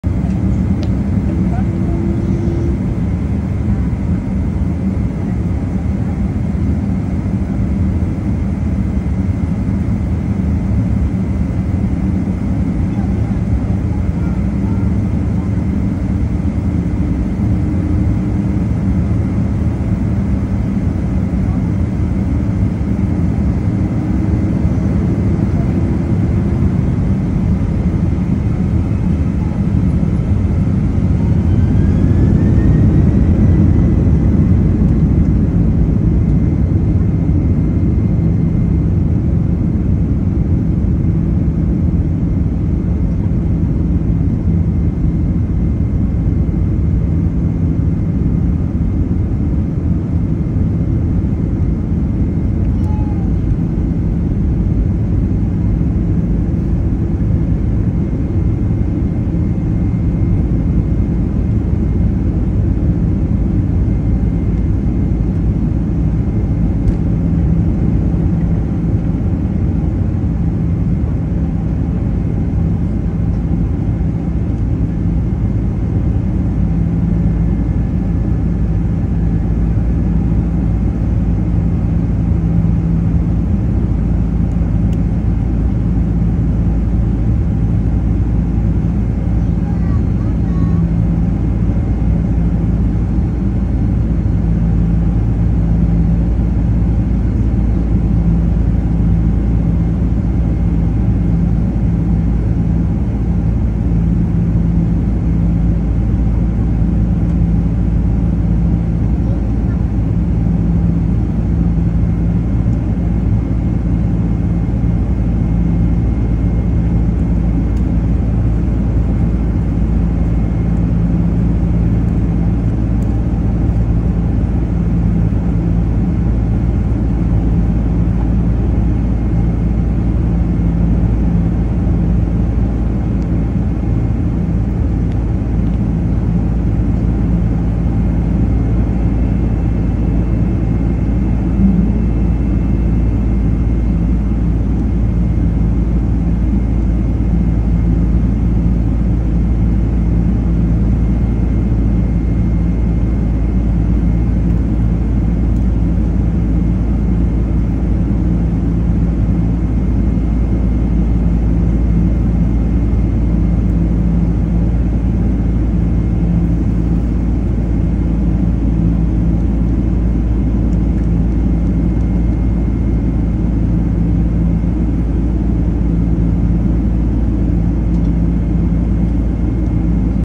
Heavy departure from Newark (KEWR) to Houston (KIAH), United Airlines Boeing 767-300.
Nice departure from Newark, NJ (KEWR) in a February winter (but warm!) afternoon.
Origin: Newark, NJ (KEWR)
Destination: Houston, TX (KIAH)
United Airlines, Boeing 767-309